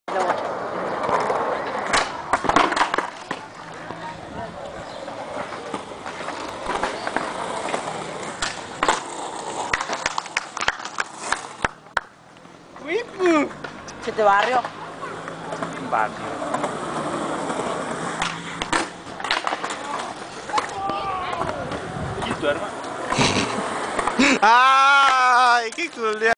Varial Flip Cantero de Rosedal
Aca un rasta flip en el gapcito de la plaza de rosedal, enjoi!